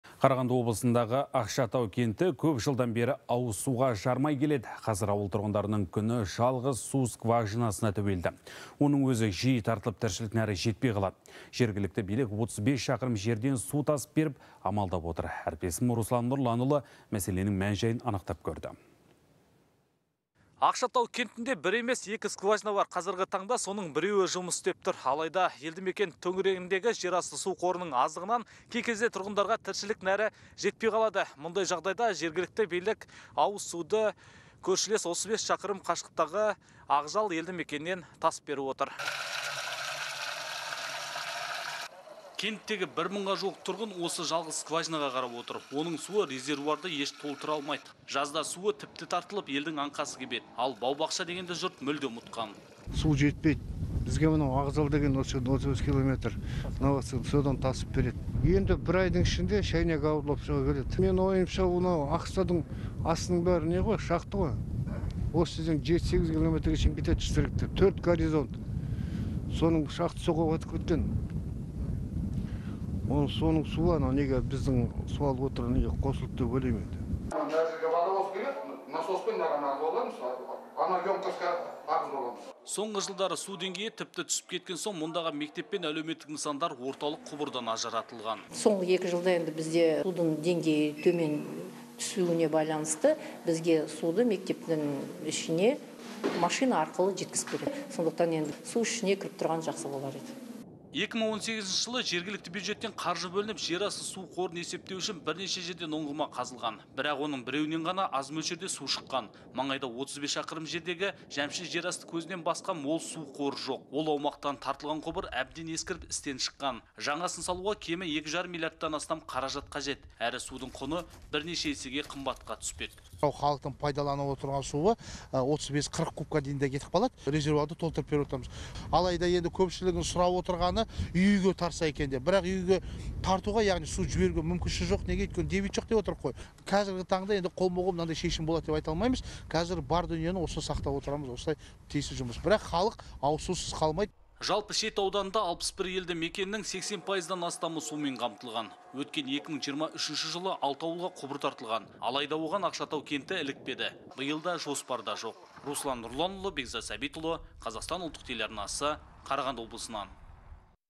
Ақшатау кентінің тұрғындары ауыз суға жарымай отыр
Қарағанды облысындағы Ақшатау кенті көп жылдан бері ауыз суға жарымай келеді. Қазір ауыл тұрғындарының күні жалғыз су скважинасына тәуелді. Оның өзі жиі тартылып, тіршілік нәрі жетпей қалады. Жергілікті билік 35 шақырым жерден су тасып беріп амалдап отыр.